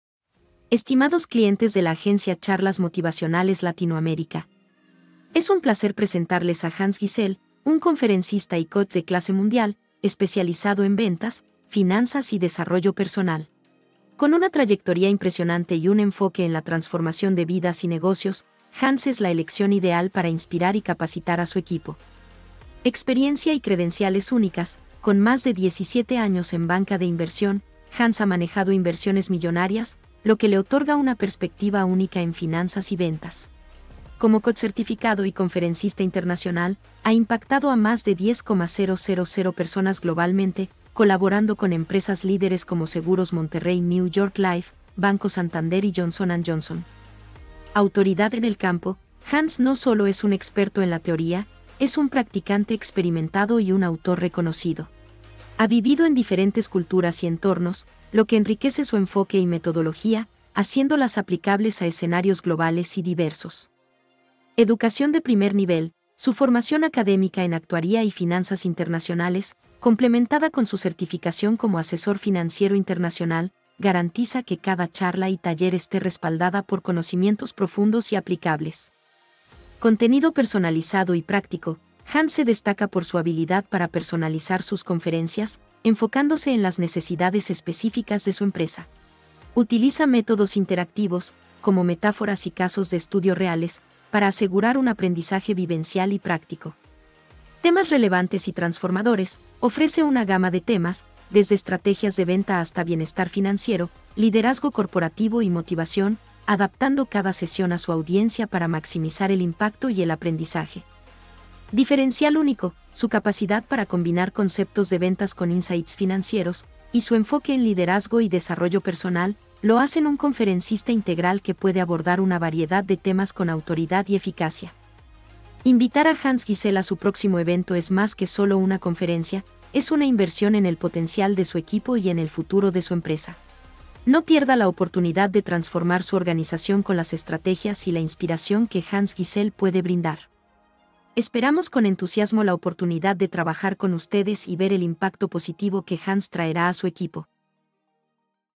¿Por qué contratar a Hans Gysel? - Charlas Motivacionales Latinoamérica
Estimados clientes de la Agencia Charlas Motivacionales Latinoamérica,

Es un placer presentarles a Hans Gysel, un conferencista y coach de clase mundial, especializado en ventas, finanzas y desarrollo personal. Con una trayectoria impresionante y un enfoque en la transformación de vidas y negocios, Hans es la elección ideal para inspirar y capacitar a su equipo.

Experiencia y Credenciales Únicas: Con más de 17 años en Banca de Inversión, Hans ha manejado inversiones millonarias, lo que le otorga una perspectiva única en finanzas y ventas. Como coach certificado y conferencista internacional, ha impactado a más de 10,000 personas globalmente, colaborando con empresas líderes como Seguros Monterrey New York Life, Banco Santander y Johnson &amp; Johnson.

Autoridad en el Campo: Hans no solo es un experto en la teoría; es un practicante experimentado y un autor reconocido. Ha vivido en diferentes culturas y entornos, lo que enriquece su enfoque y metodología, haciéndolas aplicables a escenarios globales y diversos.

Educación de Primer Nivel: Su formación académica en Actuaría y Finanzas Internacionales, complementada con su certificación como Asesor Financiero Internacional, garantiza que cada charla y taller esté respaldada por conocimientos profundos y aplicables.

Contenido Personalizado y Práctico: Hans se destaca por su habilidad para personalizar sus conferencias, enfocándose en las necesidades específicas de su empresa. Utiliza métodos interactivos, como metáforas y casos de estudio reales, para asegurar un aprendizaje vivencial y práctico.

Temas Relevantes y Transformadores: Ofrece una gama de temas, desde Estrategias de Venta hasta Bienestar Financiero, Liderazgo Corporativo y Motivación, adaptando cada sesión a su audiencia para maximizar el impacto y el aprendizaje.

Diferencial Único: Su capacidad para combinar conceptos de ventas con insights financieros, y su enfoque en liderazgo y desarrollo personal, lo hacen un conferencista integral que puede abordar una variedad de temas con autoridad y eficacia.

Invitar a Hans Gysel a su próximo evento es más que solo una conferencia; es una inversión en el potencial de su equipo y en el futuro de su empresa. No pierda la oportunidad de transformar su organización con las estrategias y la inspiración que Hans Gysel puede brindar.

¡Esperamos con entusiasmo la oportunidad de trabajar con ustedes y ver el impacto positivo que Hans traerá a su equipo!